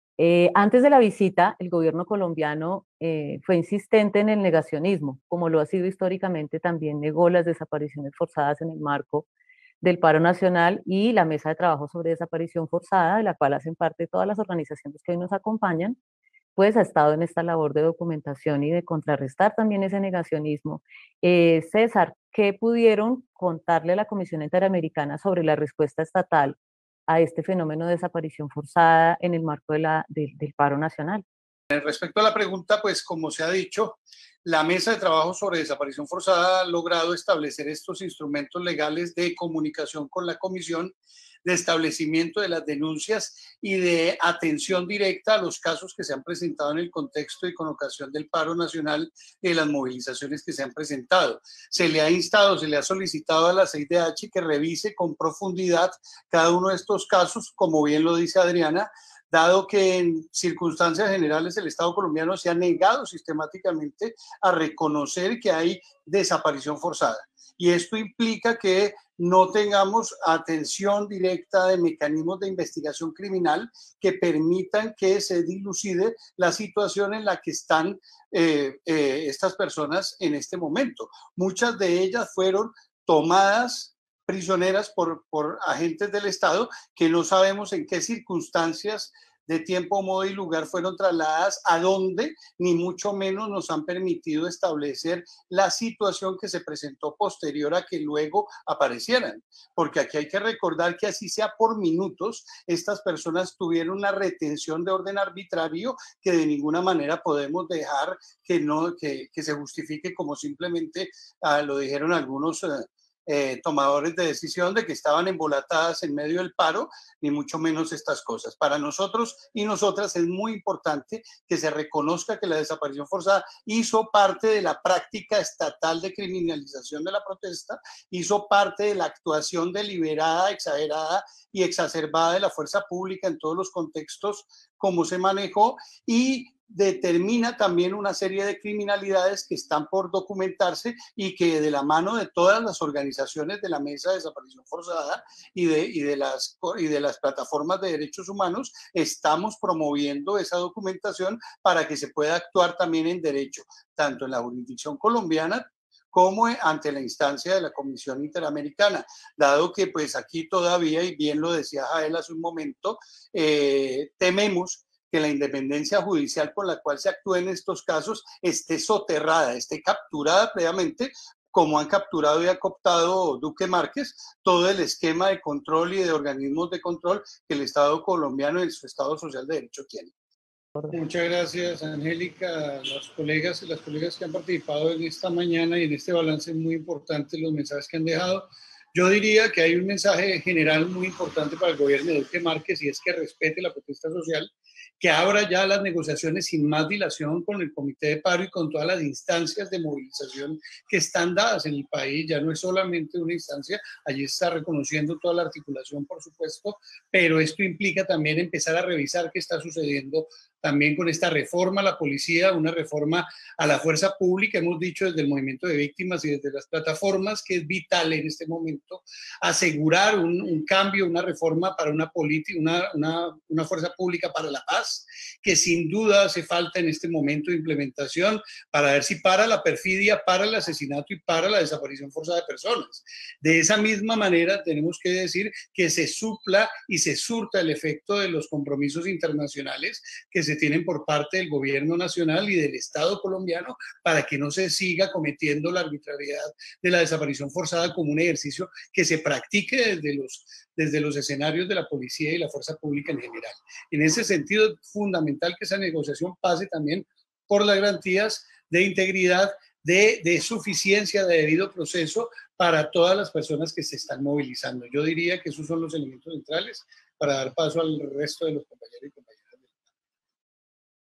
César Santoyo, OFB: ¿Qué pasó en el encuentro entre la CIDH y la sociedad civil?
César Santoyo del Colectivo Jurídico Orlando Fals Borda,
presenta un balance sobre lo que fue la visita de la CIDH en Colombia.

Evento organizado por el Colectivo de Abogados José Alvear Restrepo. Encuentra toda la conversación en nuestra fanpage